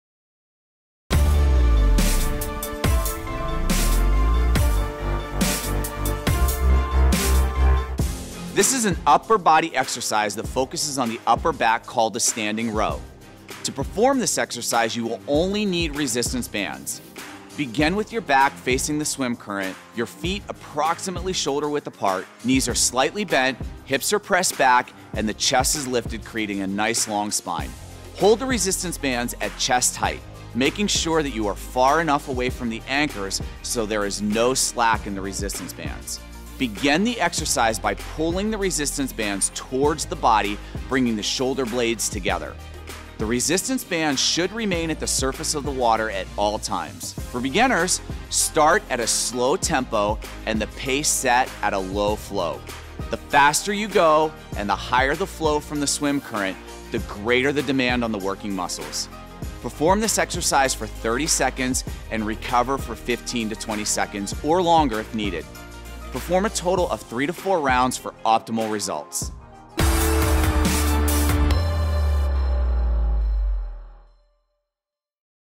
Water Workouts: Standing Rows instructional (Endless Pools Aquatic Fitness Systems)
So much more than a Swim Spa! Low-impact water workouts in the privacy and comfort of home are kinder to your joints, and allow you to gain the strength and flexibility you need.